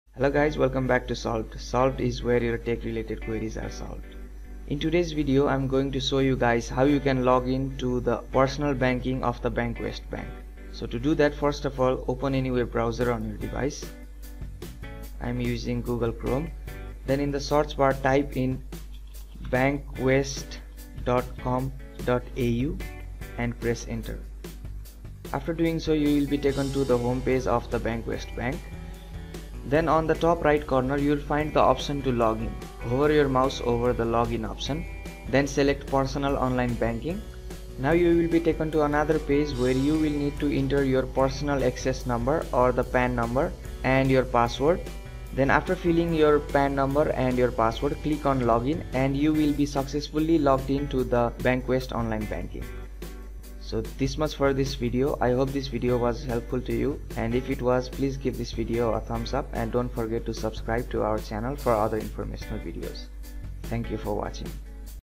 BANKWEST | How To Sign In Online Banking | Login Personal Banking Bankwest 2022
Welcome to SOLVED.
SOLVED is where we try to solve your tech related queries.

🗣This videos shows you the the steps to sign in to the personal online banking of the Bankwest Bank.

Hope this video was useful to you.
Feel free to post your queries in the comment section.